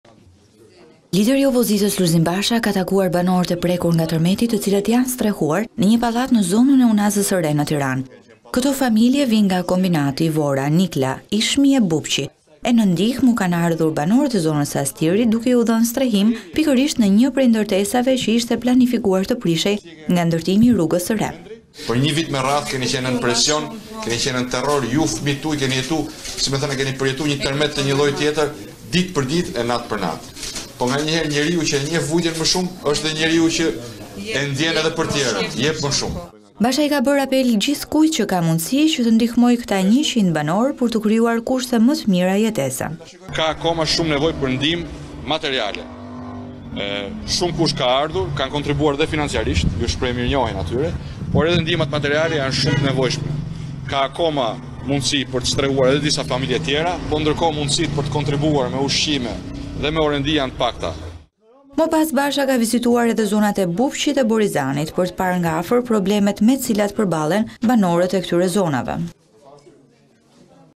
Basha: Shtëpitë u dëmtuan që në 21 shtator, askush nuk i ndihmoi - Vizion Plus
Kryetari i Partisë Demokratike , Lulzim Basha, vizitoi familjet e strehuara tek një godinë disa-kateshe në Unazën e Re.Në këtë objekt janë strehuar rreth 100 banorë të prekur nga tërmeti, mes të cilëve 30 fëmijë.Sipas selisë blu, ata ndihmohen në mënyrë vullnetare nga strehimi deri tek ushqimet dhe ilaçet, pasi nga bashkia apo qeveria nuk ka ardhur asnjë ndihmë, megjithëse janë në dijeni të qendrës që ndihmon vullnetarishr 100 banorë.Basha vlerësoi ndihmën që poi u jepet të prekurve nga tërmeti.“Kushtet mund të mos jenë optimale, por janë ofruar me zemër nga këta njerëz që kanë përjetuar një lloj tjetër tërmeti, tërmetin shtetëror”- u shpreh zoti Basha, duke iu referuar përplasjes së banorëve të Astirit kundër projektit që u shembte shtëpitë. “Po tregoni se keni një zemër të madhe, sepse e njihni vuajtjen për shtëpinë”- tha Basha.PD në njoftimin për mediat thotë se disa prej banorëve, të zhvendosur nga Kombinati, Vora, Nikla, Ishmi dhe Bubqi treguan se janë lënë të braktisur jo vetëm pas këtij tërmeti, por edhe pas tërmetit të 21 Shtatorit.Sipas Partisë Demokratike, gjatë takimit me familjet e strehuara tek Astiri, doli në pah shqetësimi serioz i ftohjes së fëmijëve nga lënia për 2-3 ditë në çadra të papërshtatshme.“Ilaçet janë siguruar deri tani nga vullnetarët, por kujdesi mjekësor i specializuar duhet të mos mungojë.Falenderoj mjekët që, në mënyrë vullnetare, po kontribuojnë për shëndetin e shume prej tyre që janë të sëmurë kryesisht për shkak të qëndrimit në çadra në ditët e ftohta dhe në orët e para të fatkeqësisë dhe po kështu dua të falënderoj nga zemra këdo që ka mbështetur me kontribute, me ushqim, me orendi.Dua të theksoj se ky është një kontribut i gjithi vullnetar.Kam një mesazh për këdo që ka mundësi të ndihmojë.Këtu ka nevojë për ushqime, pajisje, tavolina, karrike, ka shumë fëmijë që po trajtohen vullnetarisht nga psikologë dhe studente të psikologjisë.